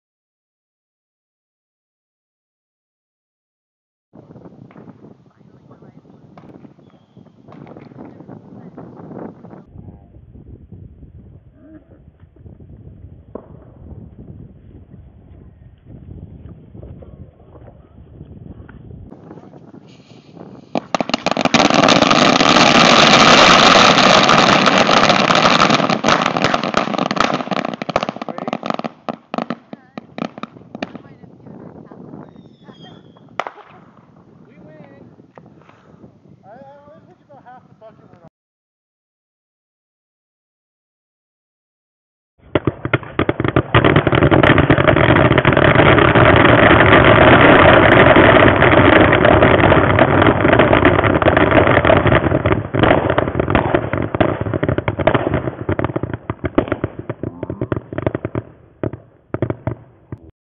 Bucket O Death
1 bucket, 2 bricks of firecrackers, 15 M150's and some ground blooming flower fireworks to get it all going.